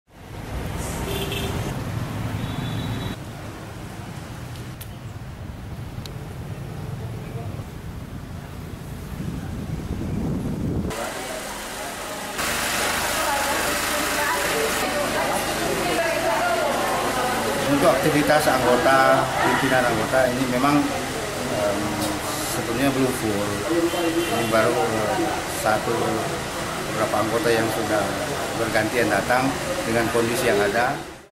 Gedung DPRD Masih Dibersihkan, Agenda Sidang Tertunda
Manado: Pascabanjir bandang seluruh pegawai DPRD Kota Manado masih berupaya membersihkan ruangan-ruangan di dalam Gedung DPRD Kota Manado termasuk ruang sidang paripurna yang terendam hingga dua meter. Banyak dokumen dan aset-aset lainnya yang rusak terendam lumpur. Selain ruangan yang beluim siap, masih banyaknya anggota DPRD yang berhalangan masuk kerja mengakibatkan sejumlah agenda sidang pun terpaksa ditunda.